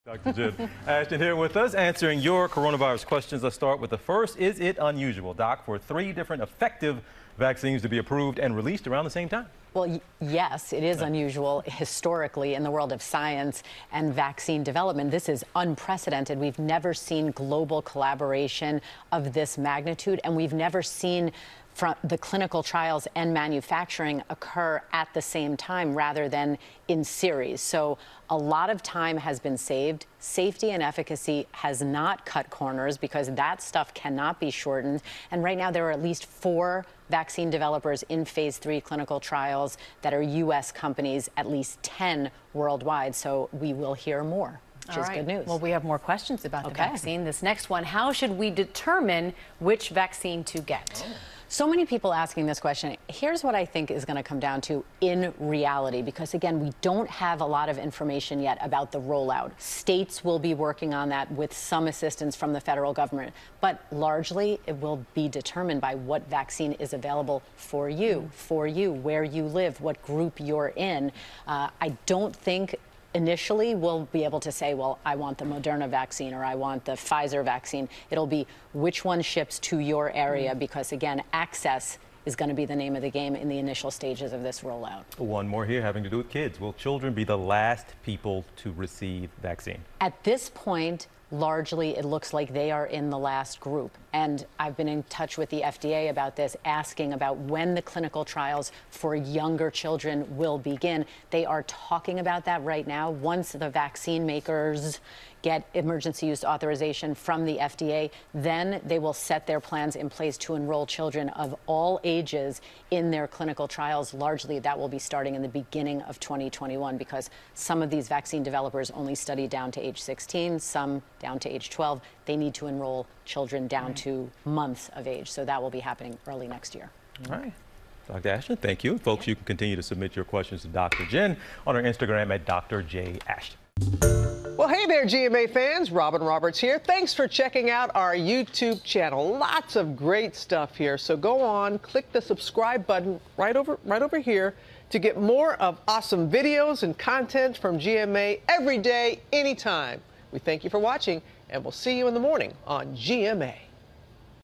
Will children be the last ones to get a COVID-19 vaccine?
Answers to viewers’ coronavirus questions from Dr. Jen Ashton.